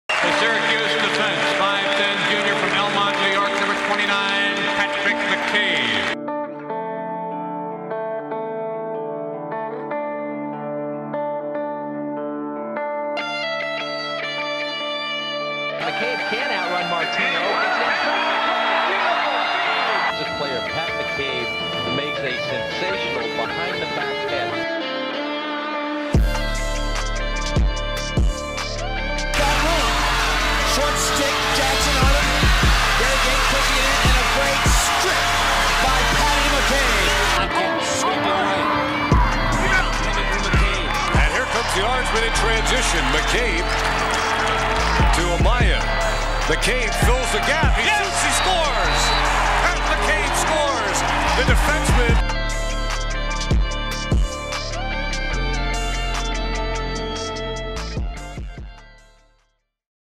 Pat McCabe - Powell Lacrosse Legends
Pat McCabe is always on the short list when you ask someone to name the top defenseman of all time.

Pat took the lacrosse world by storm with his takeaway checks and athletic lockdown defense for the dominant Cuse teams of the late 80s. He played pro lacrosse for 15 years in the NLL and MLL and had a great international career.

Pat is 4x All-American, Defensemen of the Year, 3x National Champion, 3x Team USA member, Captain of the New York Saints, Captain of the Long Island Lizards, and a member of the National Lacrosse Hall of Fame.

Pat had uncanny awareness, was an unreal ground baller, silky smooth, a winner, a leader, and made the defense position look sexy. He is one of the most respected players in our sport and you can’t find a more humble superstar in our game. He is now an incredible coach for the Adelphi women's team. We loved the times we got to be teammates with him and the battles we had when we played against him. #29, thank you for being such an amazing ambassador for lacrosse!

Powell Lacrosse salutes Pat McCabe!

Explore The Field